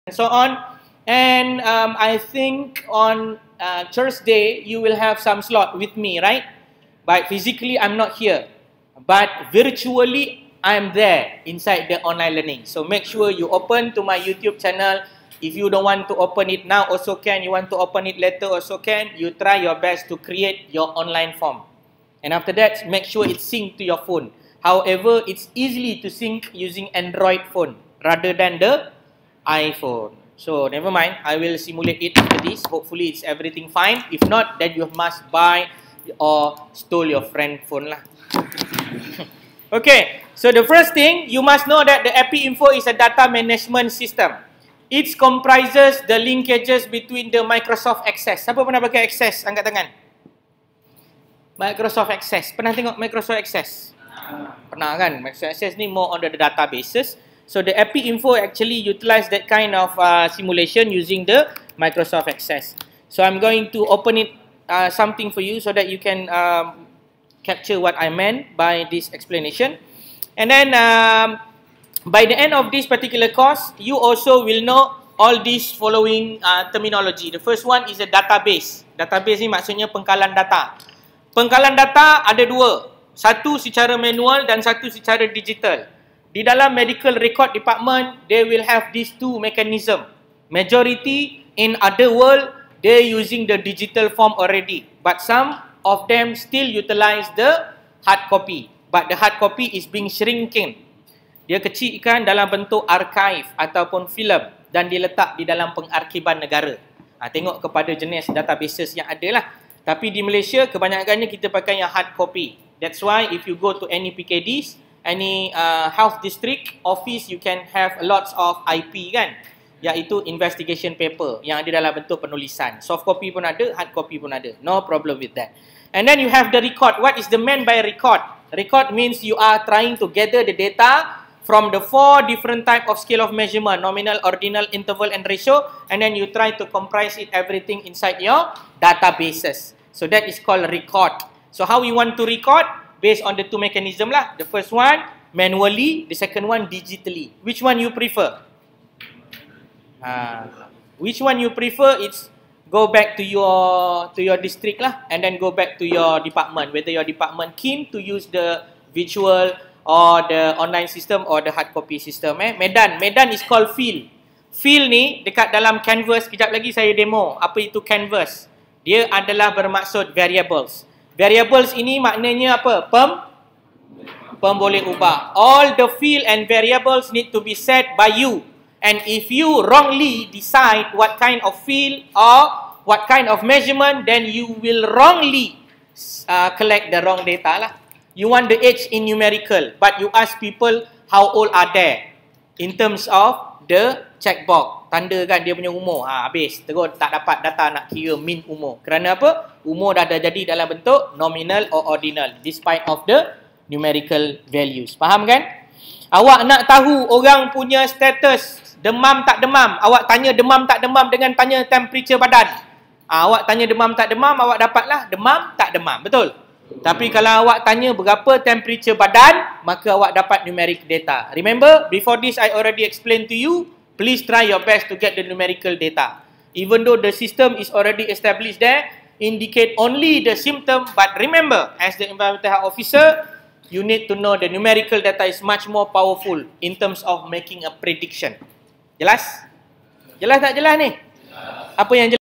#1 using epi info as electronic tool for data collection and analysis in epidemiology investigation
This video explain the steps in developing online questionnaire and data analysis. This workshop was conducted and aimed to help professionals understanding the SPSS data analysis technique and the tricks behind the speedy accurate medical statistics. Interpretation and how to explain about the findings of the statistical output also being explain in this video.